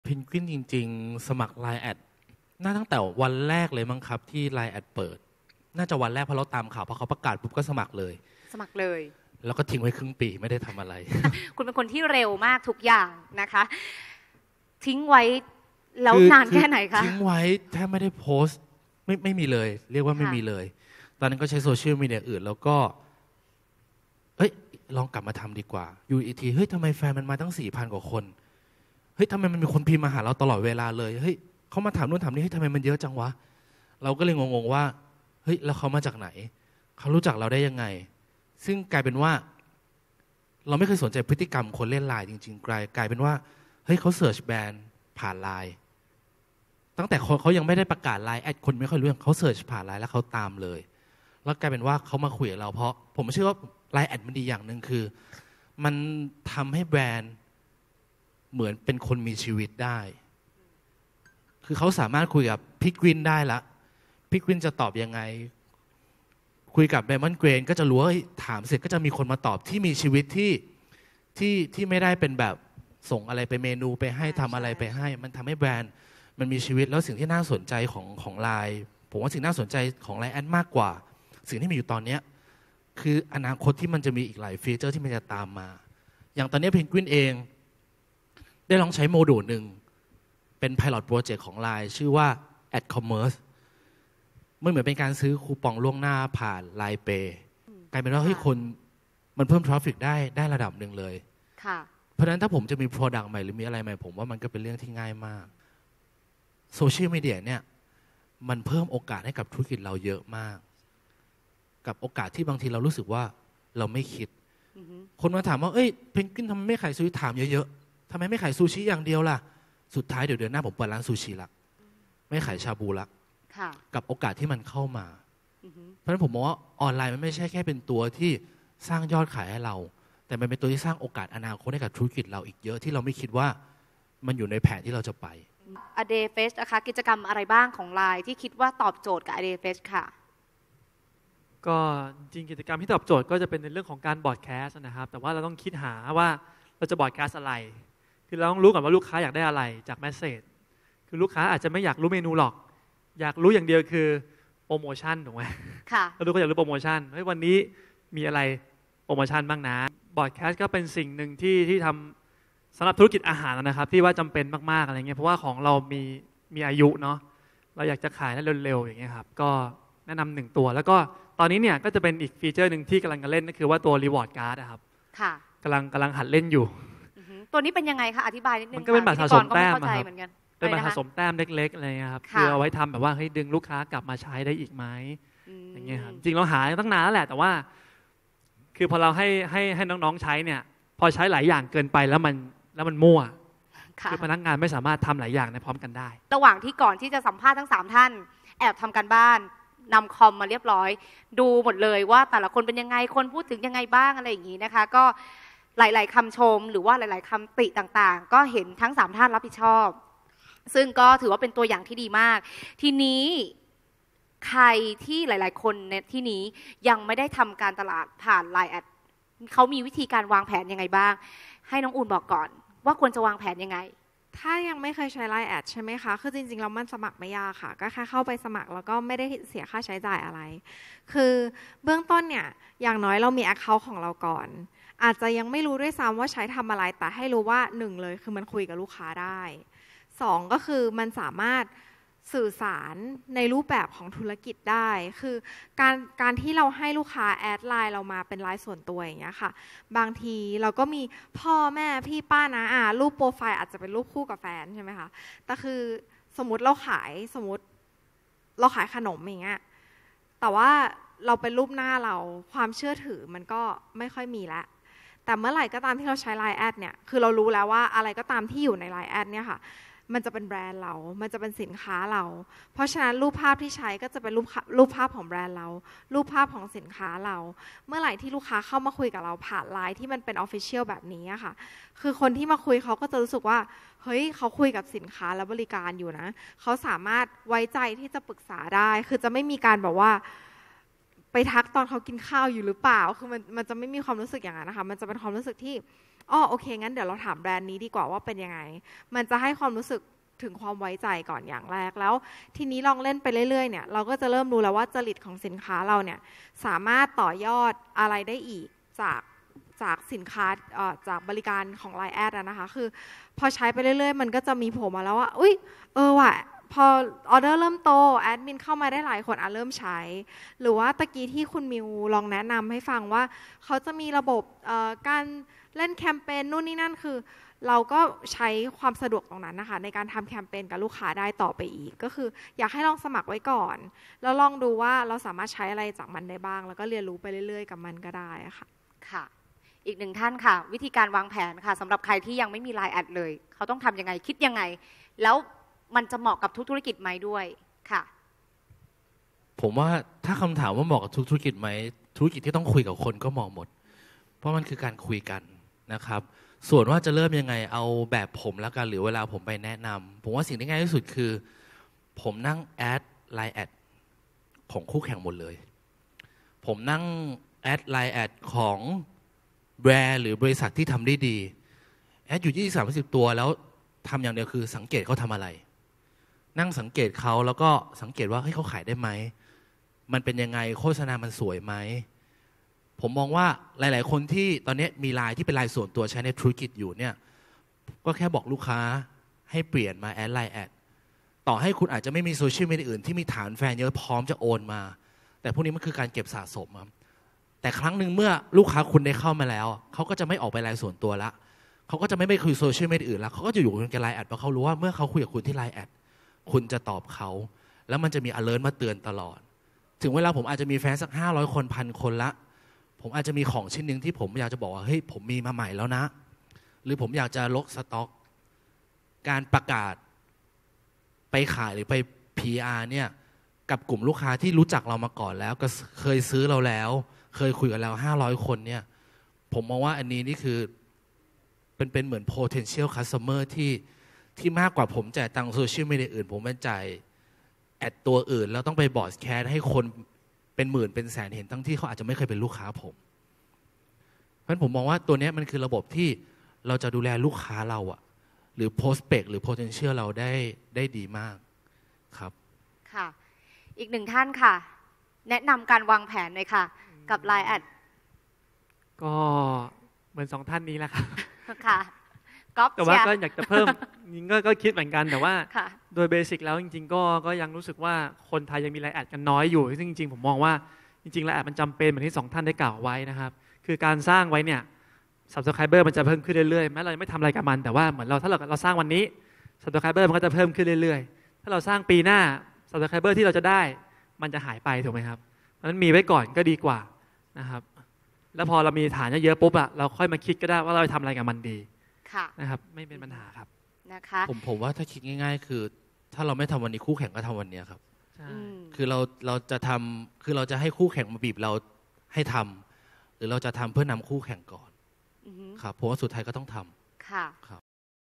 เพิ่มช่องทางการตลาดด้วย LINE@ กับธนาคารกรุงเทพ (ตอน 3)
สุดยอดงานสัมมนาสำหรับ SME
ที่จะทำให้ธุรกิจคุณเพิ่มยอดขายหลายเท่าด้วยการใช้ LINE@
ในการเพิ่มช่องทางการตลาด

พิเศษสุด พบกับ คุณอริยะ พนมยงค์ กรรมการ
บริษัท LINE ประเทศไทย ที่จะมาพูดในหัวข้อตอบโจทย์ SME
ยุคใหม่ด้วย LINE

และพบกับ 3 สุดยอดธุรกิจ
aDay Fresh  l Penguin Eat Shabu  l Diamond Grains
ที่จะมาเผยเคล็ดลับความสำเร็จในการเพิ่มยอดขายด้วย LINE@

ติดตามงานสัมมนาดีๆ ตลอดทั้งปี ได้ที่